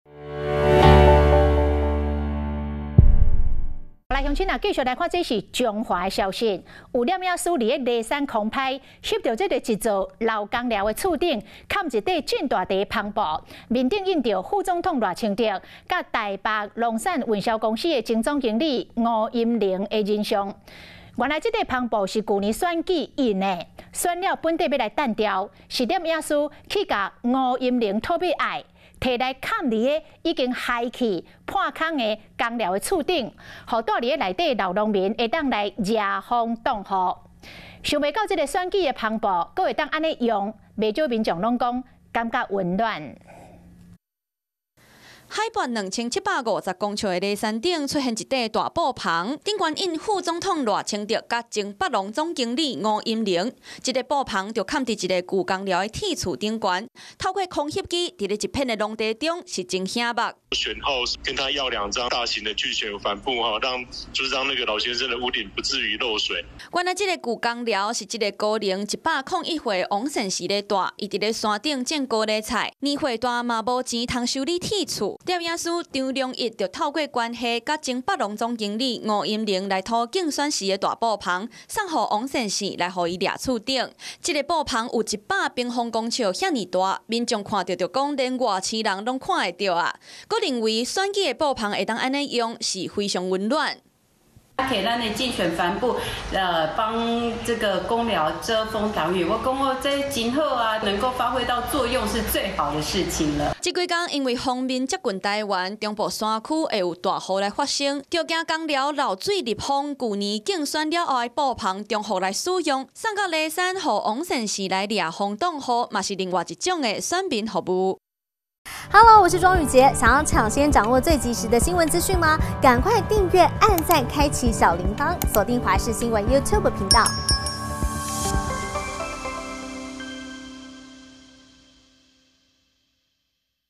賴清德.吳音寧「競選帆布」 蓋住工寮為農遮風擋雨｜華視台語新聞 2024.04.26
另類的選民服務，有攝影師在梨山空拍，拍下一座老舊工寮的屋頂上，蓋了一塊巨型帆布，上面印著副總統賴清德，和前北農總經理吳音寧的人像，這塊帆布是去年選舉印製的，選完原本要丟掉，是攝影師特地跟吳音寧要來，蓋在破掉的工寮屋頂上，幫住在裡面的老農夫遮風擋雨，因為帆布非常大塊，不少民眾看了覺得很震撼，外星人都看得到，更認為，選舉帆布還能這樣用，很溫暖。